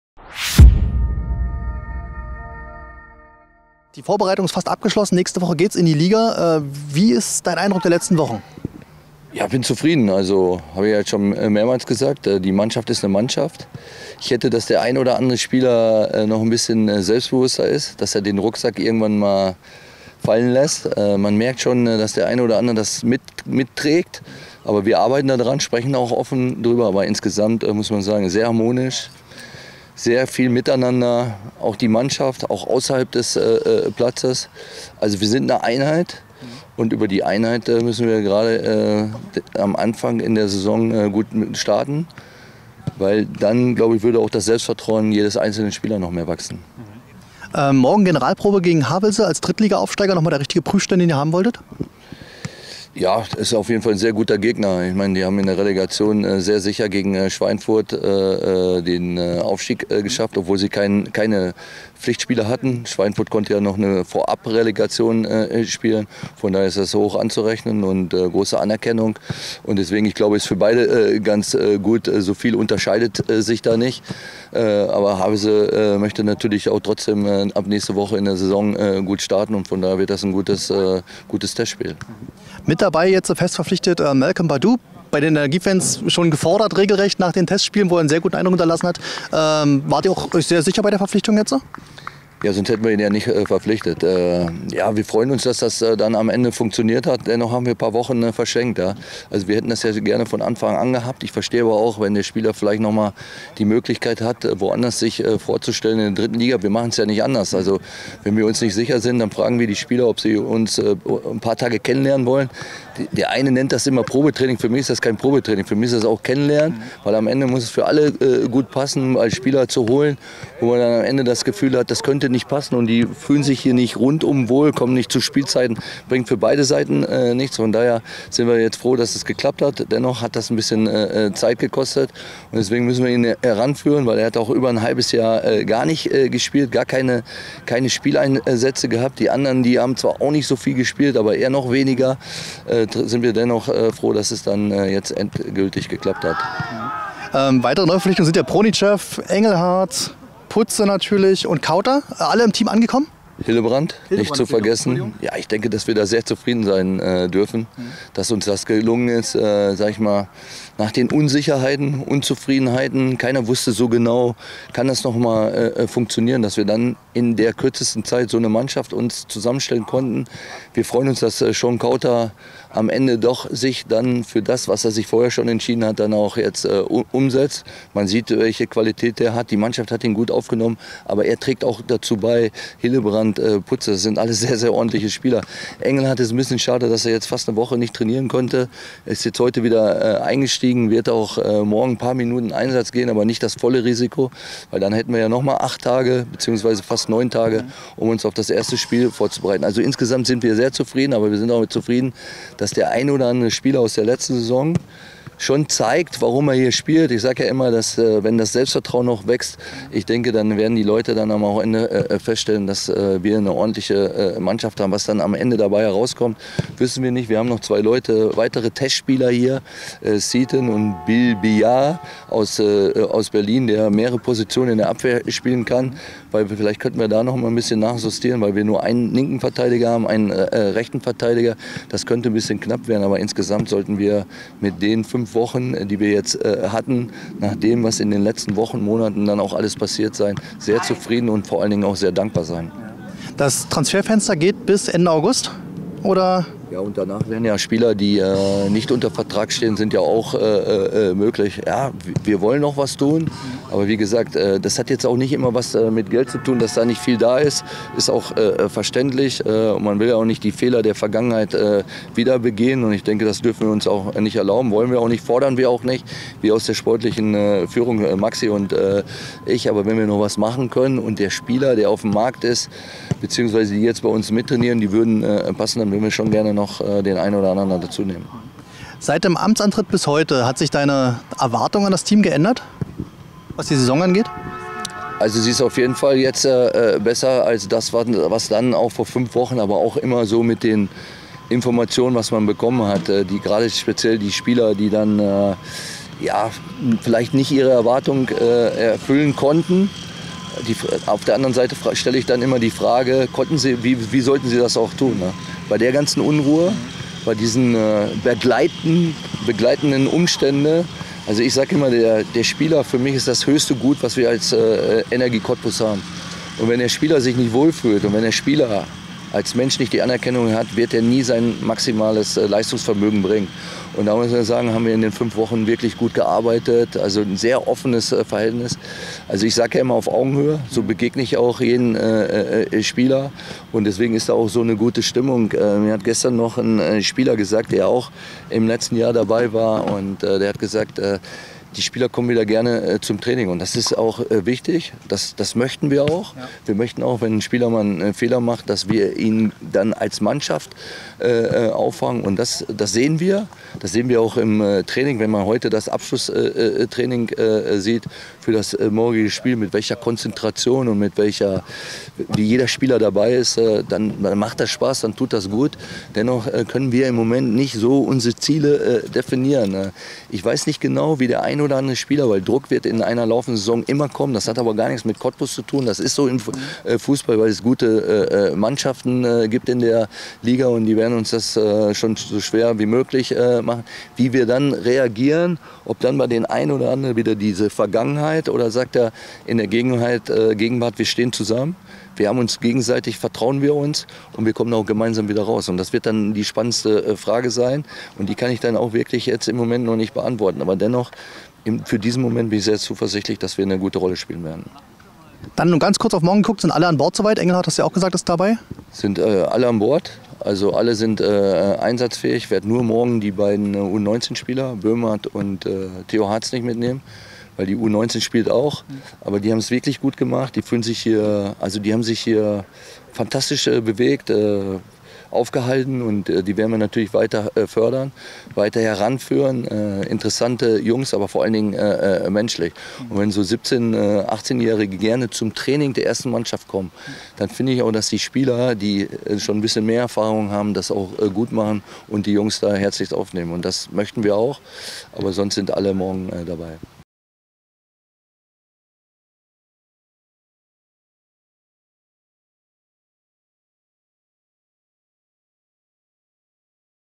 ⚽FC Energie Cottbus | Trainer Pele Wollitz über Vorbereitung, Team, Saisonstart und TSV Havelse
Pele Wollitz spricht über die Vorbereitungsphase auf die Saison beim FC Energie Cottbus, die Stimmung im Team, die bisherigen Neuverpflichtungen Engelhardt, Hildebrandt, Pronichev, Putze, Badu und Kauter, weitere Probespieler und die Generalprobe gegen Drittligaaufsteiger TSV Havelse.